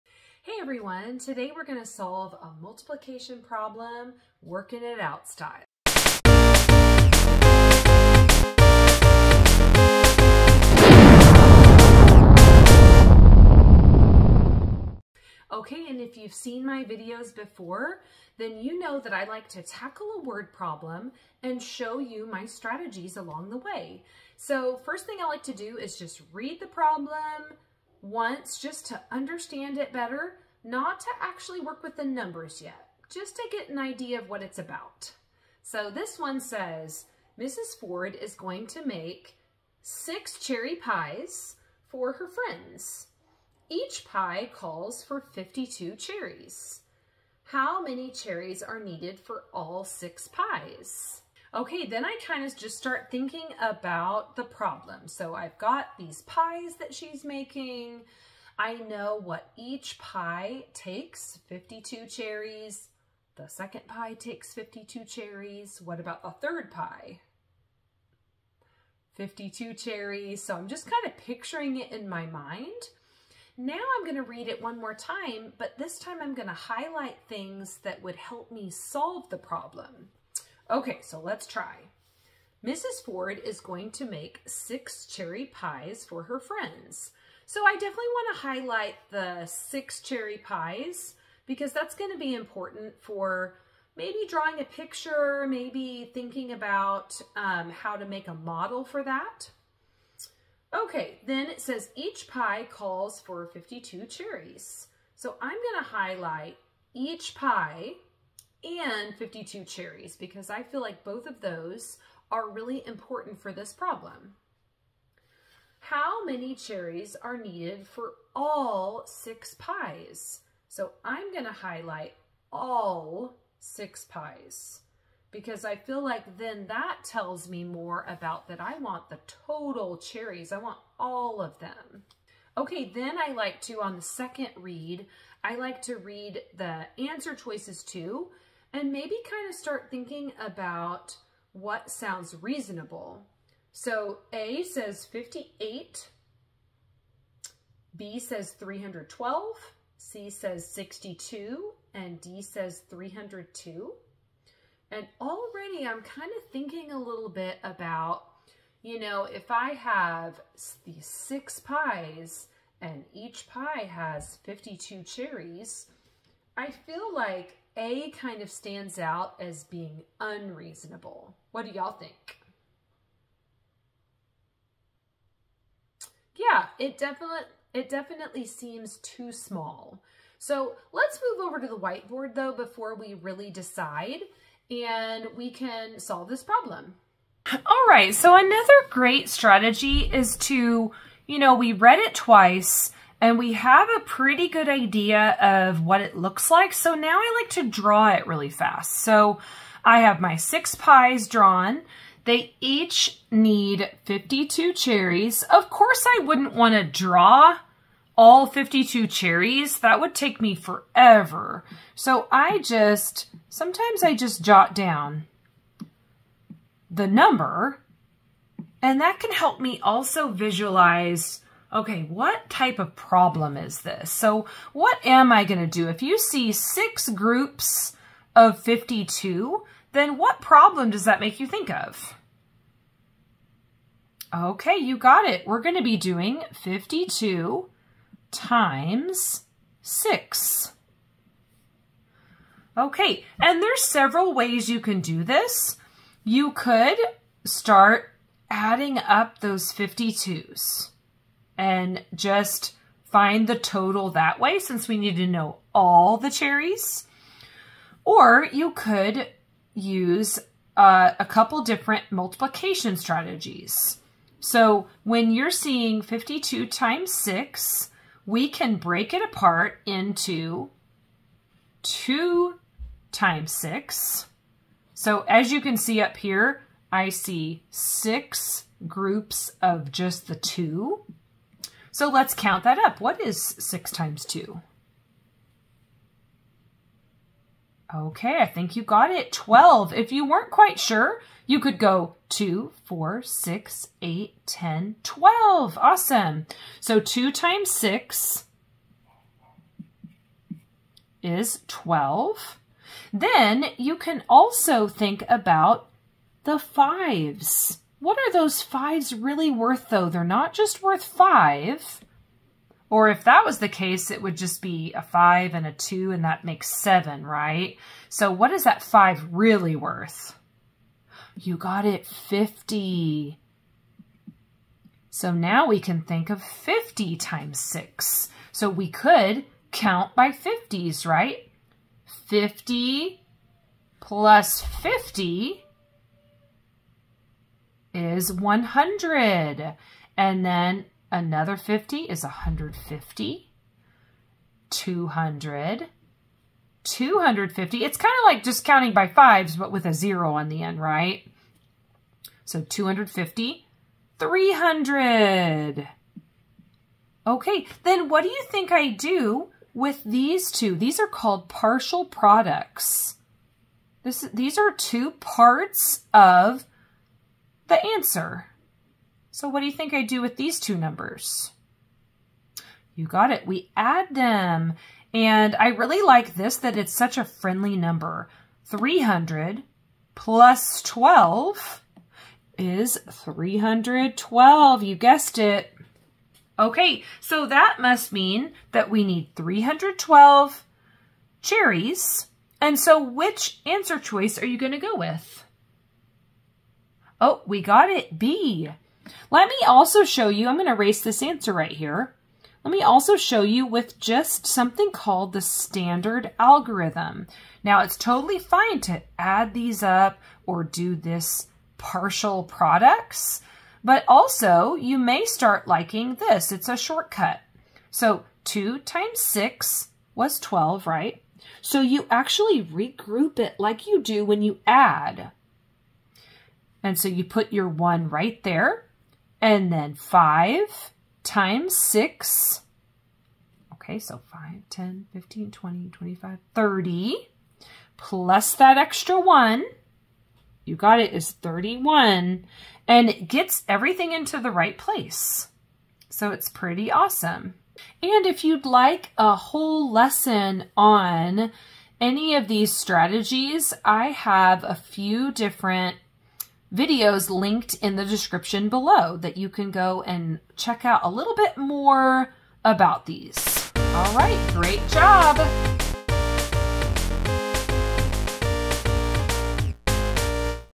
MULTIPLICATION with 2 DIGIT by 1 DIGIT [3.4G] Problem Solving Strategies: 3rd Grade Math
Learn how to tackle a multiplication word problem using my strategies!  I'll walk you through my steps that I always take and you can feel confident with your problem solving skills!

Here's some full video lessons on multiplication if you'd like to start here: